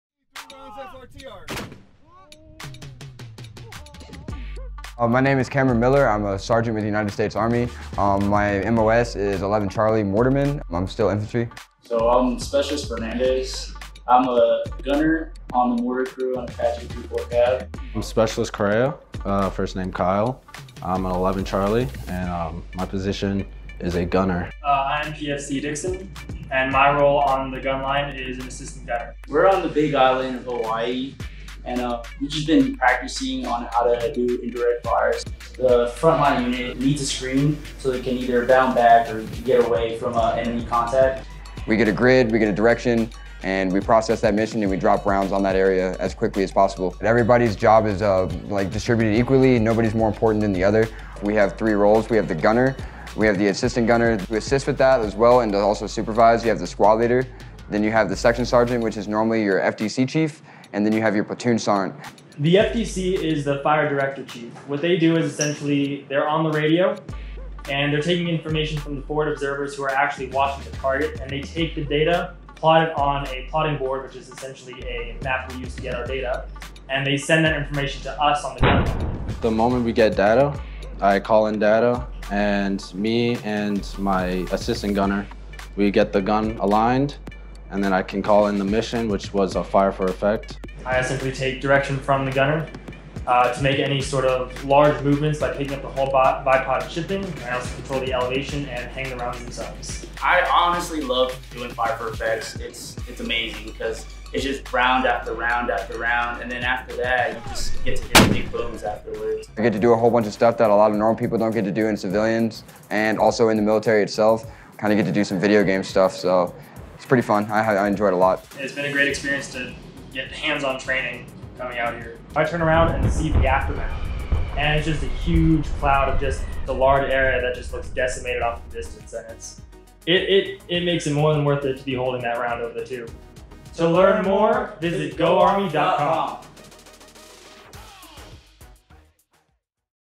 Mortar Firing in the Army | GOARMY
Mortar firing in the Army requires teamwork, leadership, humility, and hard work. Are you ready to join a team that's bigger than you? Find your skillset and purpose in environments like this. ☝️ Ready to know where you fit in?